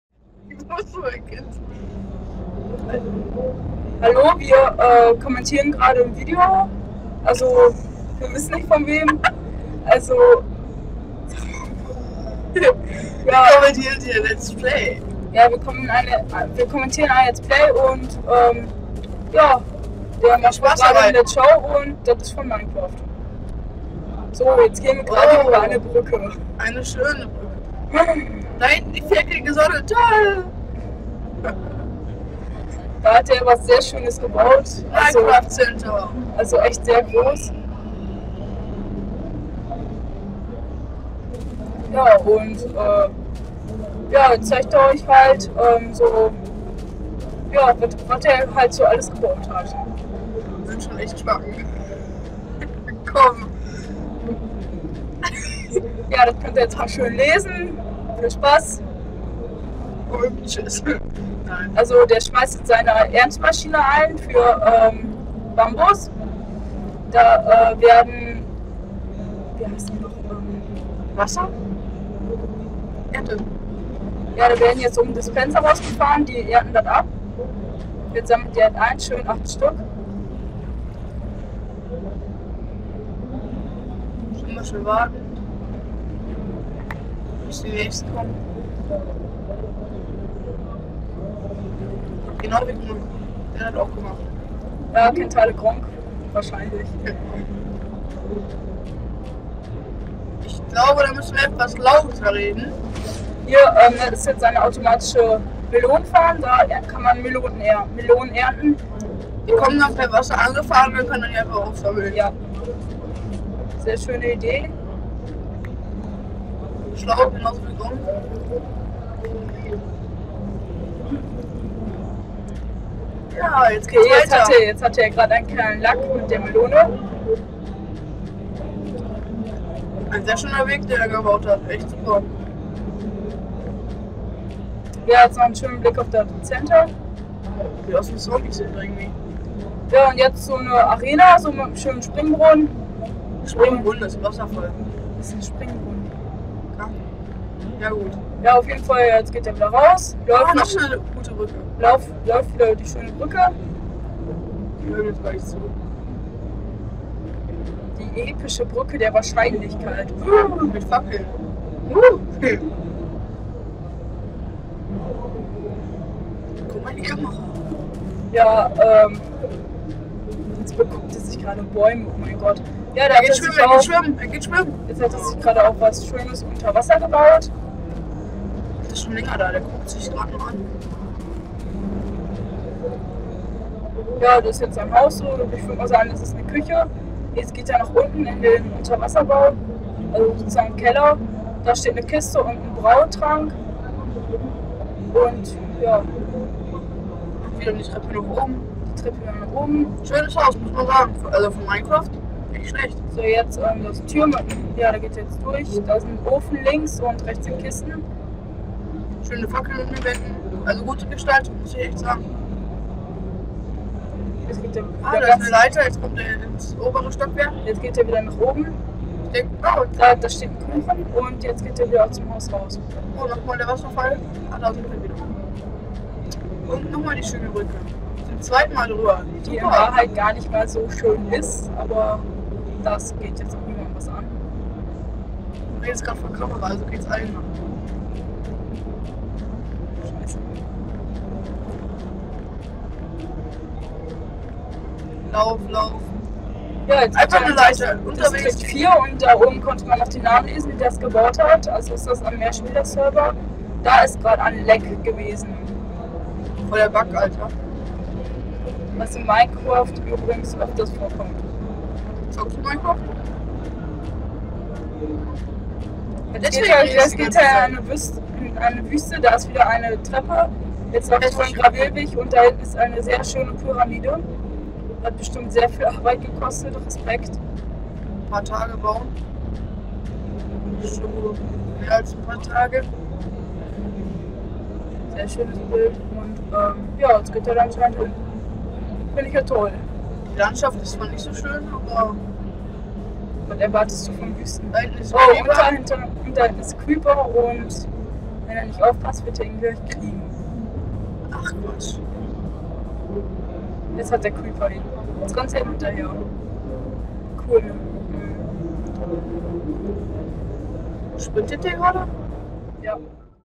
LP Karaoke TN79
Video des Projekts "Let´s Play Live" auf der gamescom 2013

Mit freundlicher Unterstützung von elgato und Logitech Let´s Play Karaoke auf der gamescom 2012